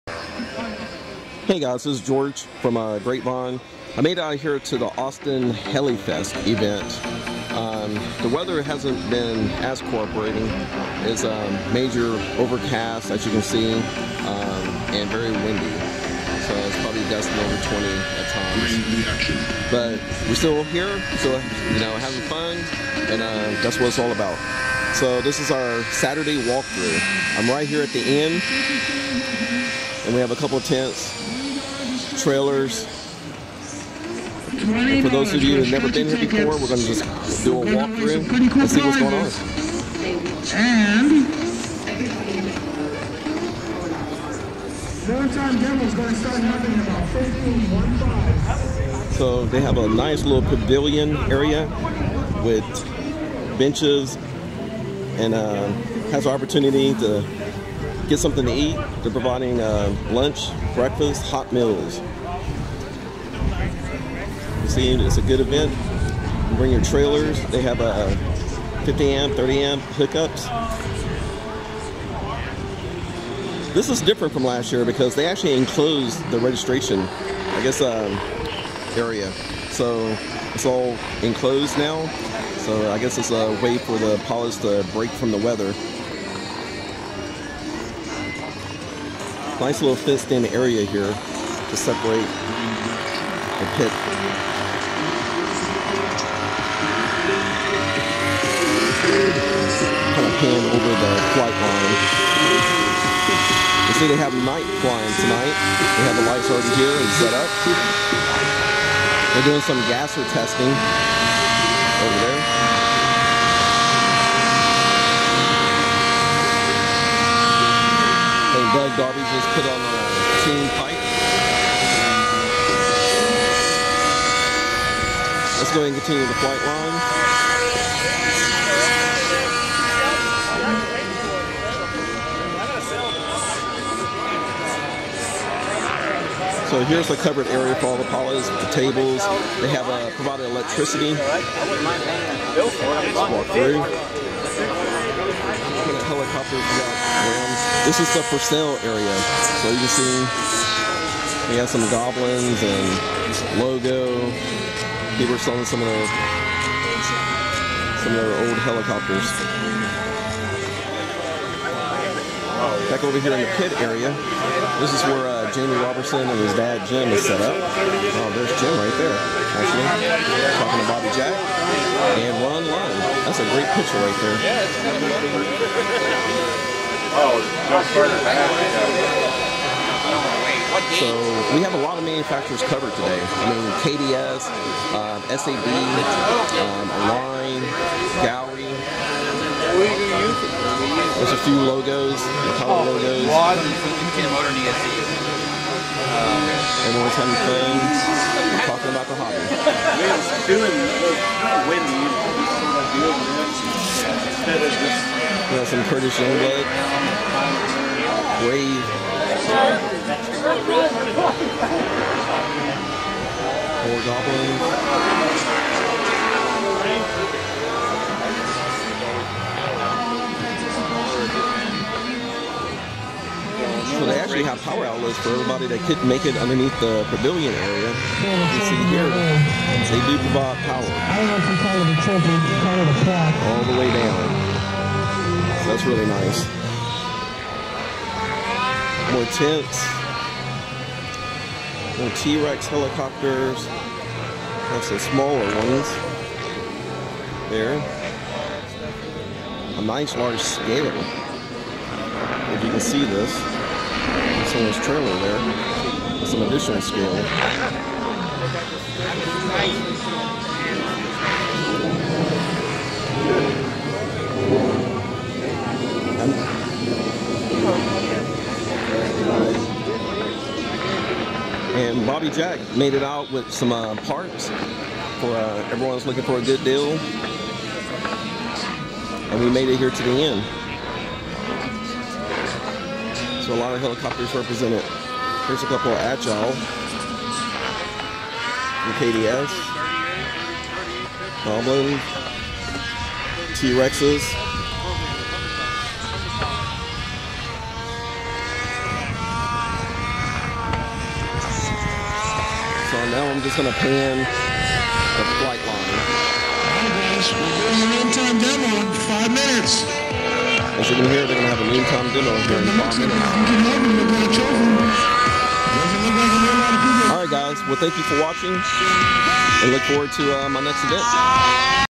George Brown Austin Helifest 2014 Fun Fly Saturday Walk Thru
George Brown Saturday flight line walk thru o fthe 2014 Austin Heli Fest Fun Fly